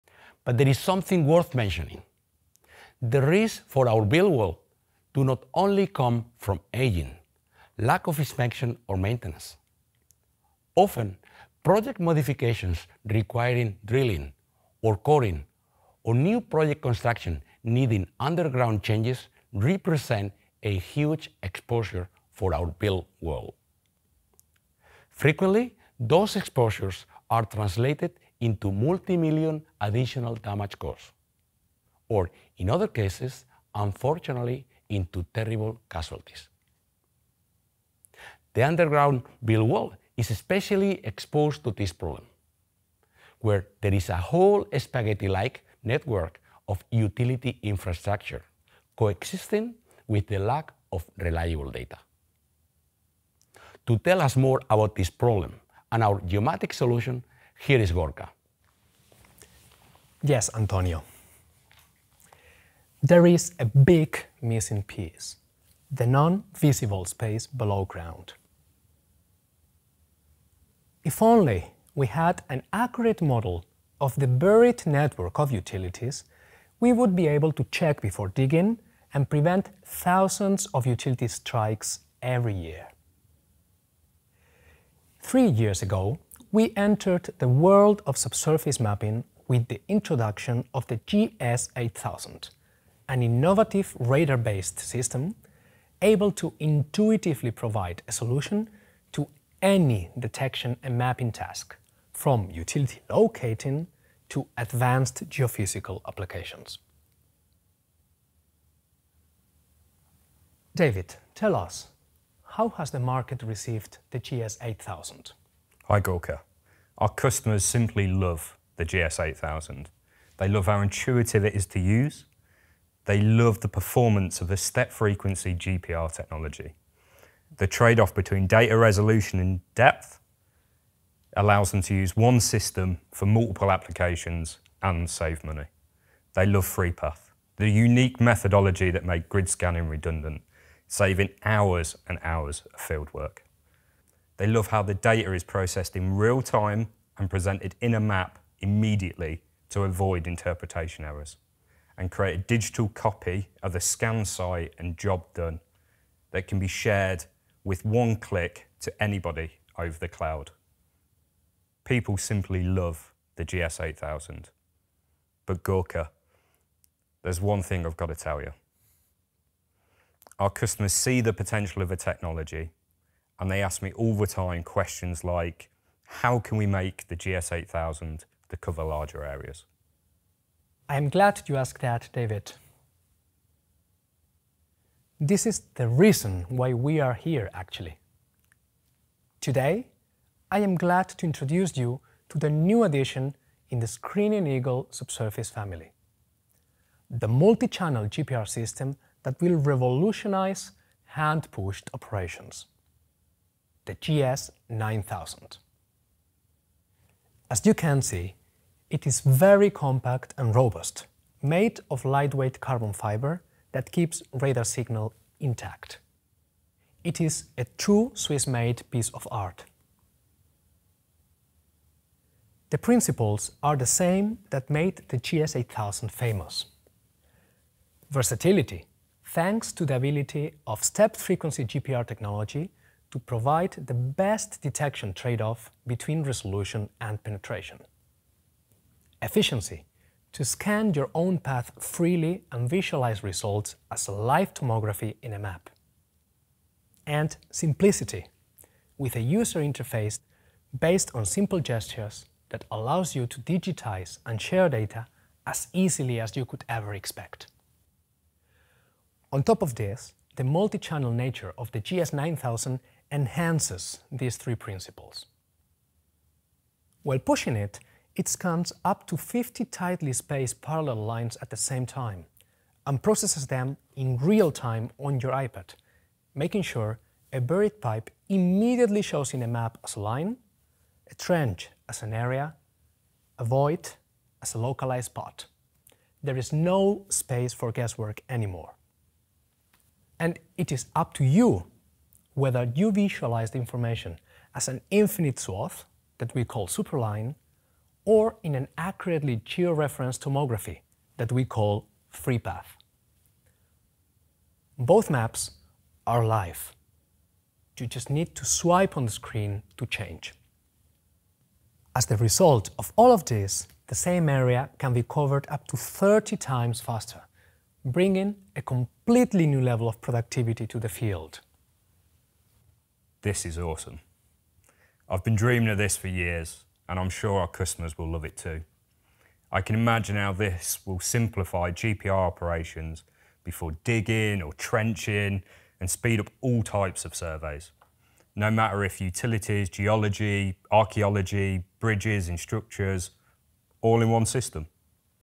Multichannel Subsurface Mapping with Real-time 3D Visualization
Meet one of Screening Eagle’s new addition to their subsurface mapping GPRs, the GS9000 Multichannel system with real-time 3D visualization, officially launched at KEY23SET – The Built World Tech Keynote.

The new GS9000 3D GPR system provides the most versatile solution to a big challenge of the underground built world: lack of reliable data for subsurface utilities such as water, gas mains, fiber optic lines and much more. See how the new multichannel solution solves this challenge with its huge data collection capabilities.